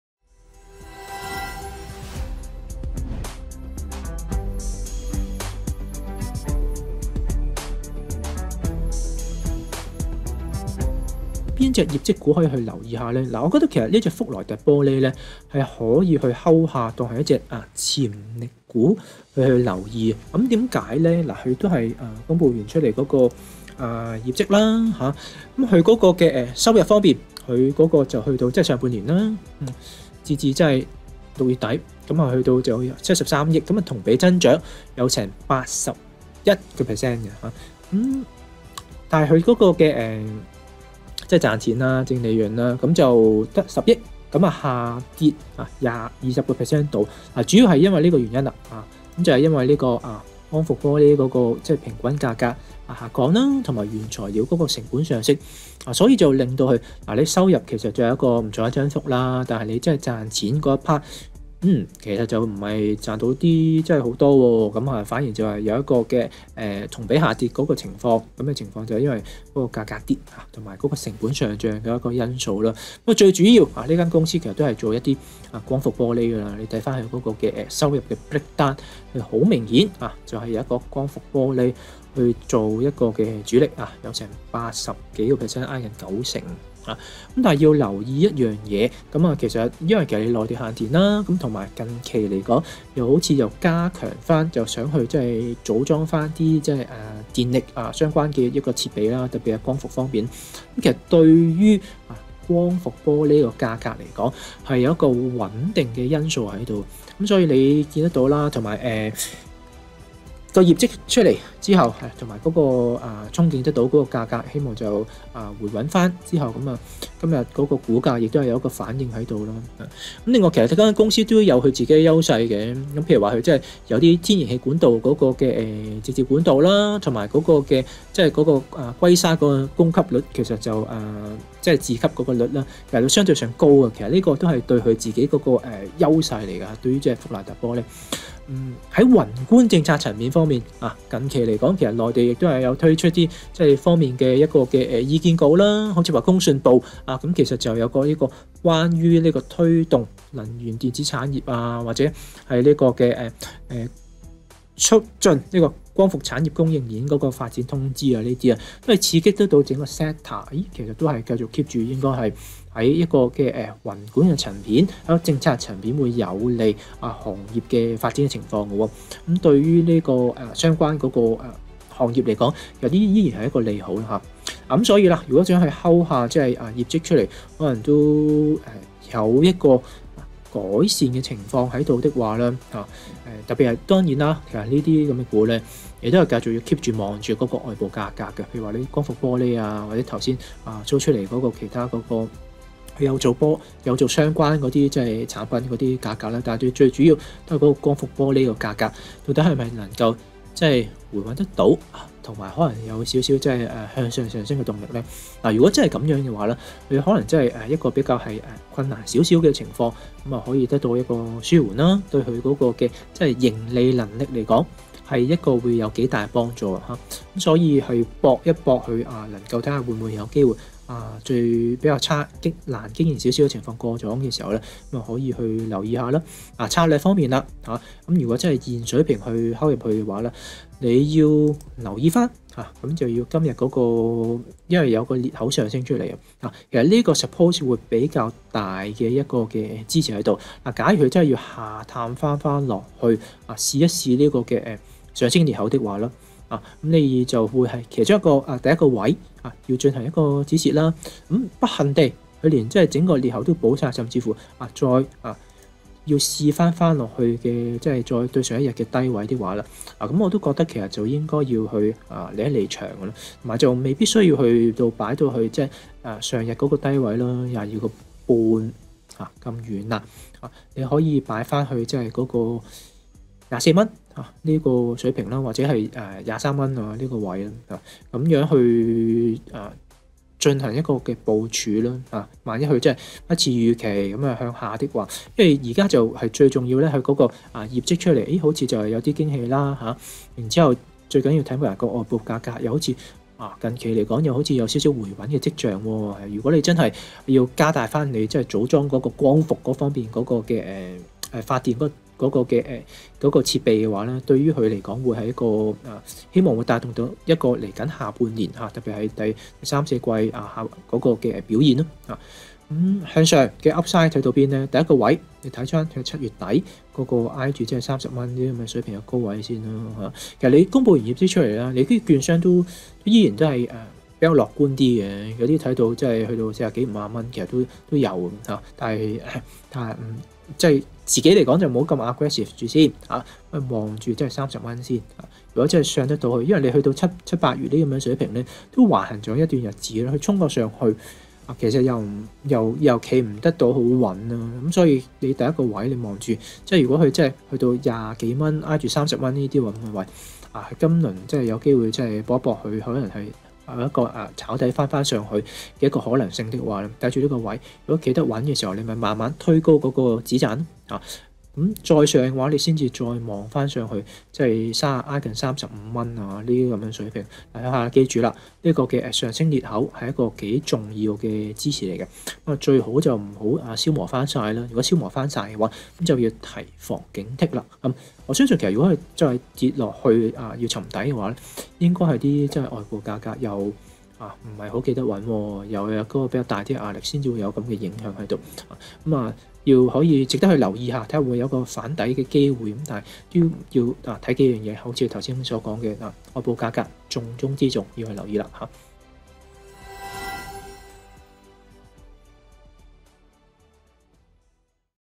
【潛力股精選】梁耀康：福萊特玻璃（06865）
福萊特玻璃（06865）收市報26.05元，升9.92%。福萊特玻璃以經營光伏玻璃為主，業績顯示上半年收入同比增長81%。獨立股評人梁耀康指，內地限電及加強組裝電力設備，為光伏玻璃提供穩定價格因素。天然氣直接管道及硅砂自給率高，均利好福萊特玻璃前景。工信部公布推動能源電子產業，及促進光伏產業供應鏈發展通知，政策有利行業前景。若光伏玻璃價格上升，料可提升盈利能力。今日股價裂口上升，上望目標價及止蝕位？立即點擊收看／收聽。